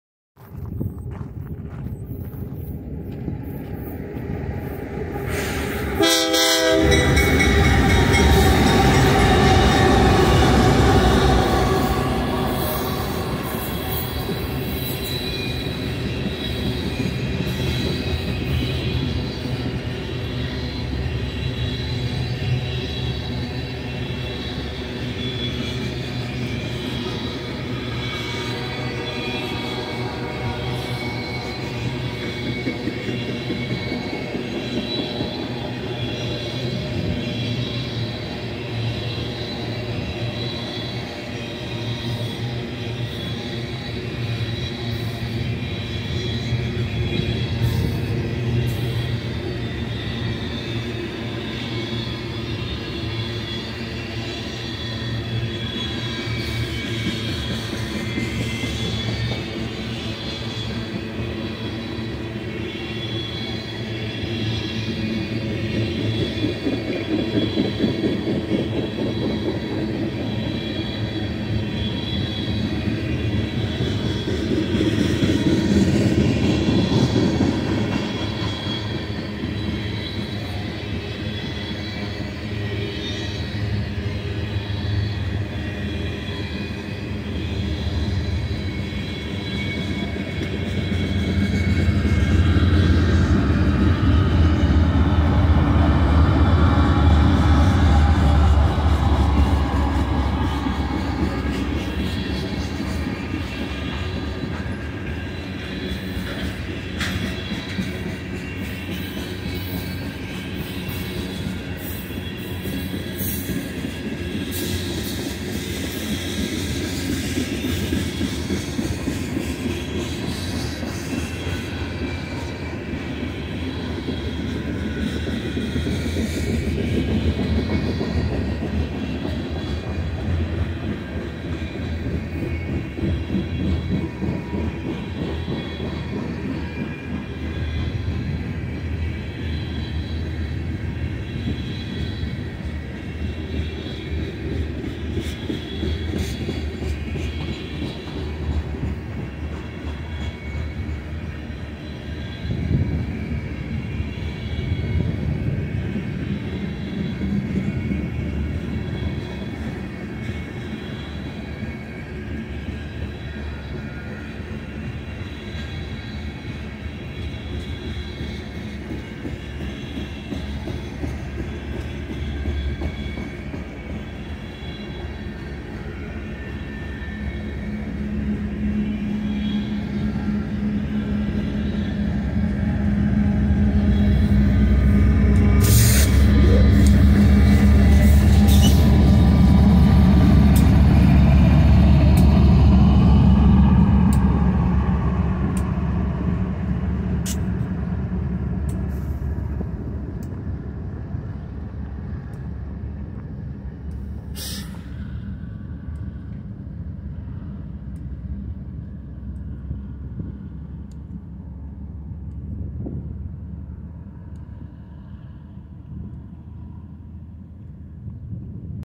Monster WB CP Grain Train Leading In Daggett Ca!
This monster westbound CP leading grain train with 3 mid DPUs and 2 rear DPUs going through here in Daggett Ca!

Here you will see in this train video is BNSF grain train like grain cars

Front Engines
8622-AC4400CW (GE) (CP) (Leading Unit)
8892-ES44AC (GE) (CP) (Trailing)
6267-ES44AC (BNSF) (Trailing)
Middle Engines
6990-ES44C4 (BNSF) (Mid DPU)
4495-AC44C6M (NS) (Mid DPU)
6818-ES44C4 (BNSF) (Mid DPU)
Rear Engines
8837-ES44AC (GE) (CP) (Rear DPU)
7398-ES44DC (BNSF) (Rear DPU)